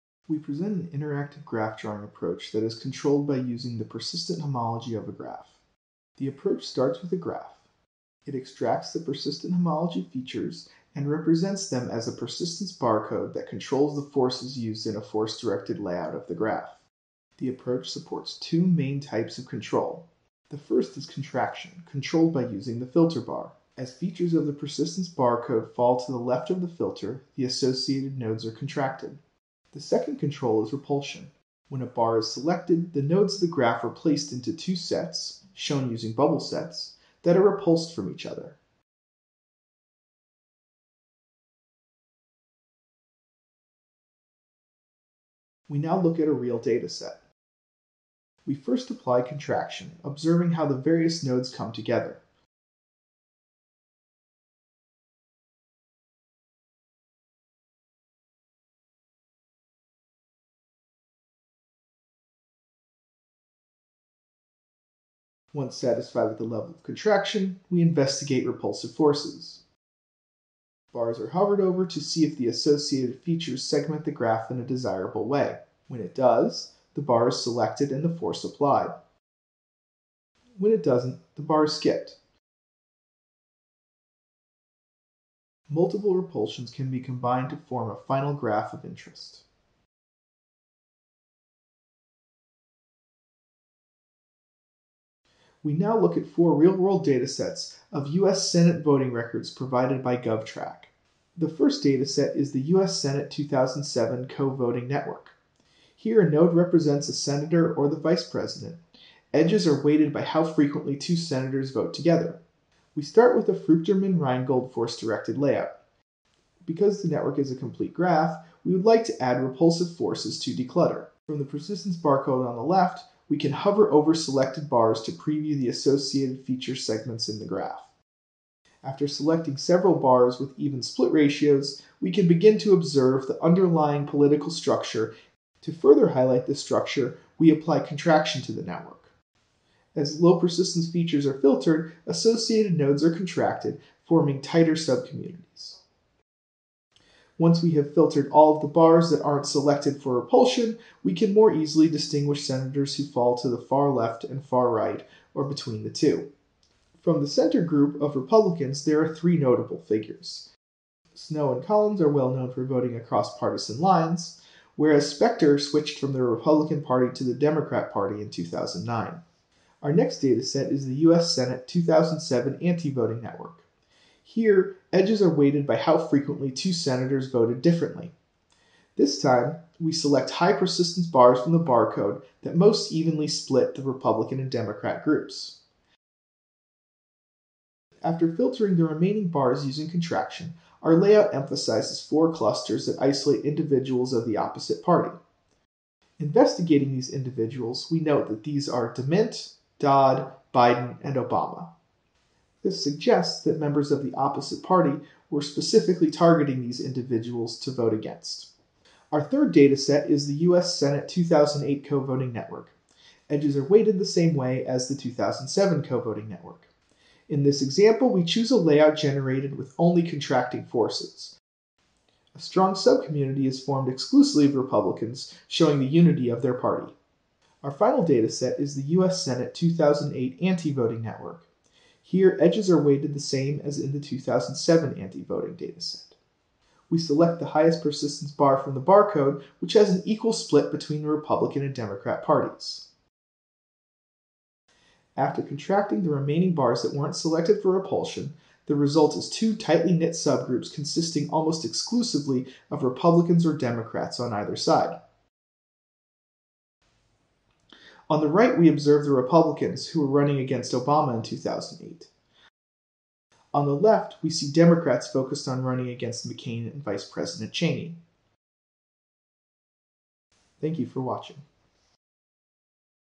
Persistent Homology Guided Force-Directed Graph Layouts
Graphs are commonly used to encode relationships among entities, yet their abstractness makes them difficult to analyze. Node-link diagrams are popular for drawing graphs, and force-directed layouts provide a flexible method for node arrangements that use local relationships in an attempt to reveal the global shape of the graph. However, clutter and overlap of unrelated structures can lead to confusing graph visualizations. This paper leverages the persistent homology features of an undirected graph as derived information for interactive manipulation of force-directed layouts. We first discuss how to efficiently extract 0-dimensional persistent homology features from both weighted and unweighted undirected graphs. We then introduce the interactive persistence barcode used to manipulate the force-directed graph layout. In particular, the user adds and removes contracting and repulsing forces generated by the persistent homology features, eventually selecting the set of persistent homology features that most improve the layout. Finally, we demonstrate the utility of our approach across a variety of synthetic and real datasets.